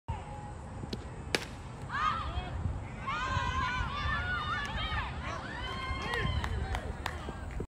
SCT SOFTBALL SUMMER EDITION: Hillsborough strikes first against Montgomery
Courtney Wengryn lifts a fly ball to right field that is dropped by Montgomery's Dana Levy and allows Katie Rygiel to score from third base to give Hillsborough a 1-0 lead in the first inning. Montgomery rallied for two runs in the fifth inning and went on to defeat Hillsborough 3-1 to advance to the finals of the Somerset County Softball Summer Edition Tournament. NJSoftball 🥎